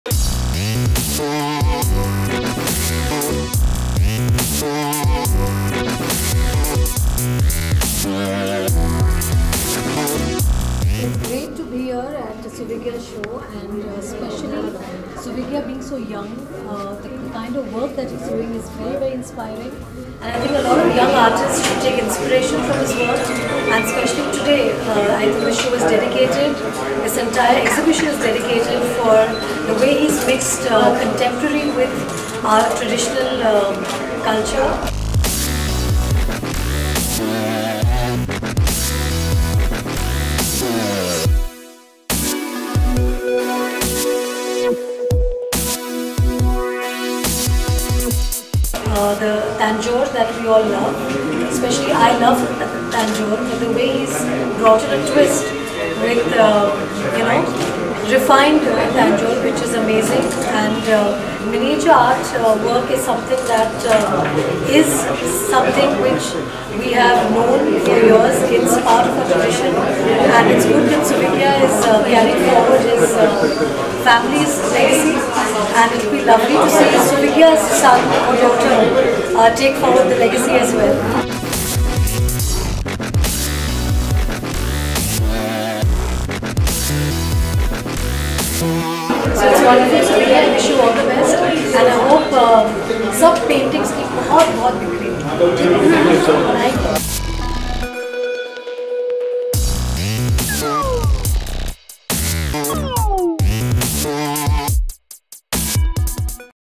Rani Mukerji Inaugurates An Art Exhibition
Rani Mukerji last movie ‘Mardaani’ had a very positive impact not only on society but also at the box-office. She was here spotted inaugurating an art exhibition which was organized by Suvigya Sharma. For more info watch this video…